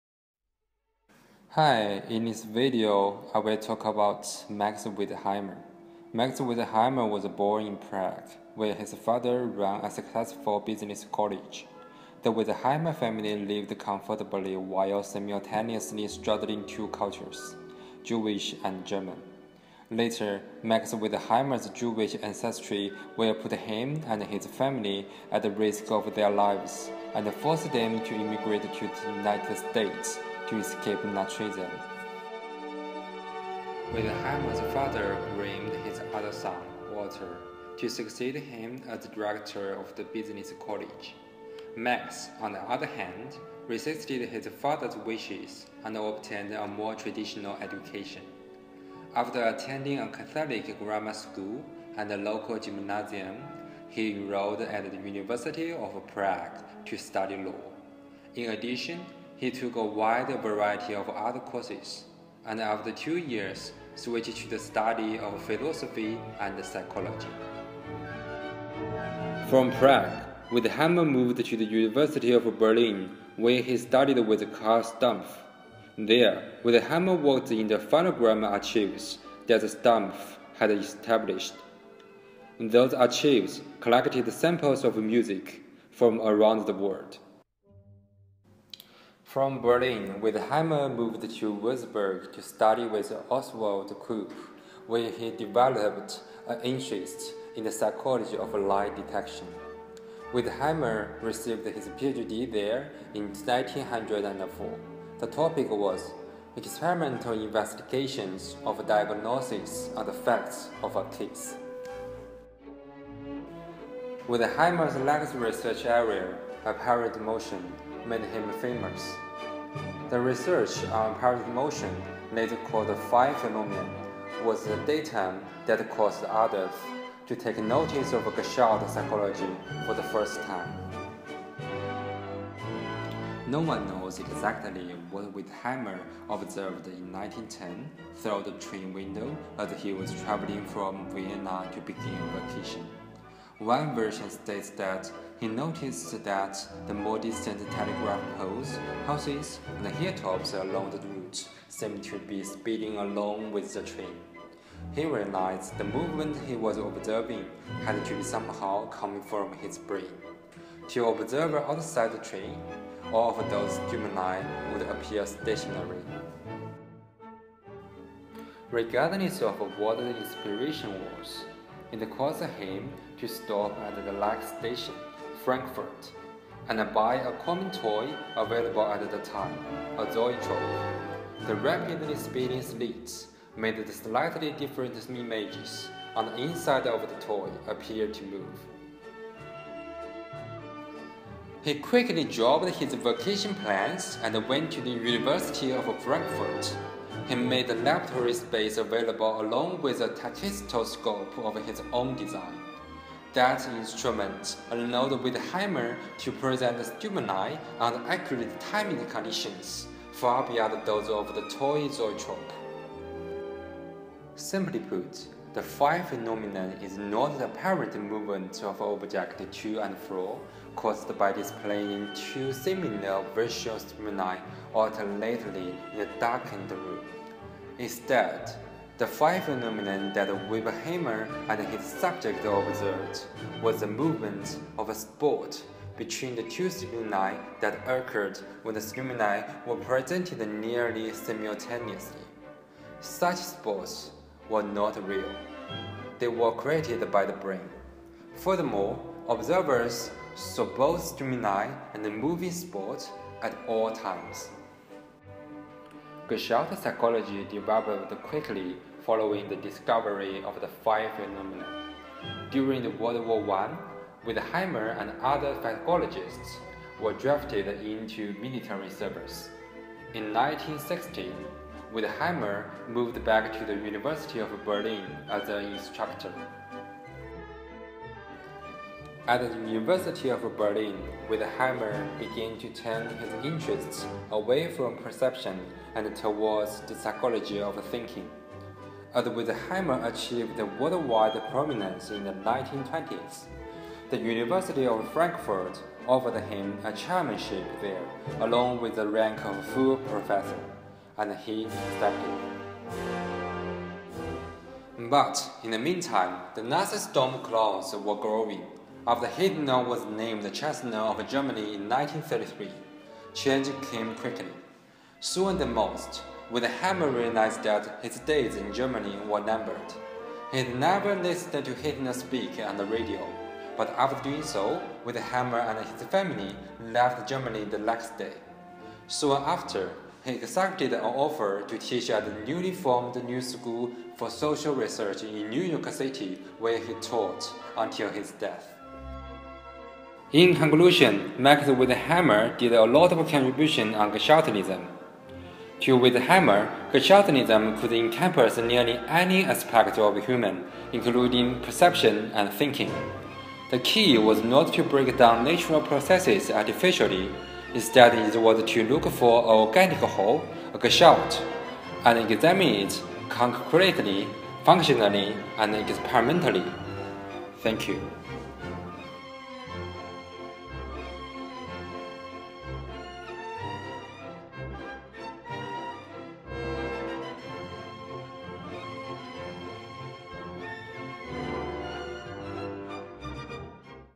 Max Wertheimer
A five minute overview of the ideas and career of Max Wertheimer, founder of Gestalt Psychology. Produced as an assignment for History of Psychology University of Waterloo, PSYCH 380 w2015